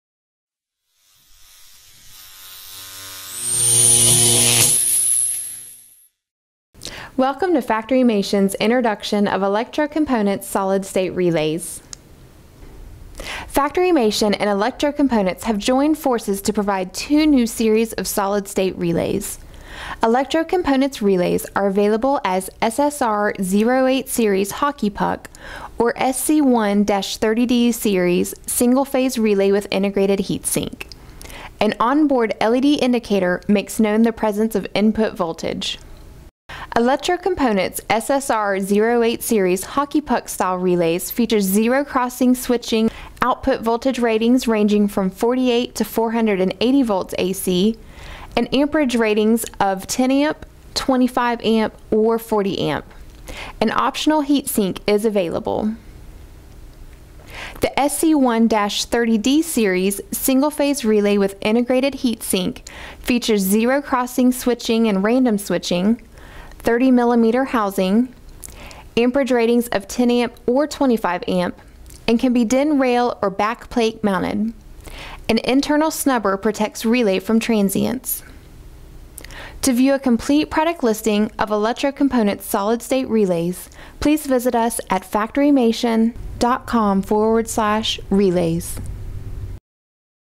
Elco Solid State Relays
FactoryMation and Electro Components have joined forces to provide two new series of solid state relays. Electro Components line of solid state relays supply customers with an expanded selection rich in functionality and diverse features.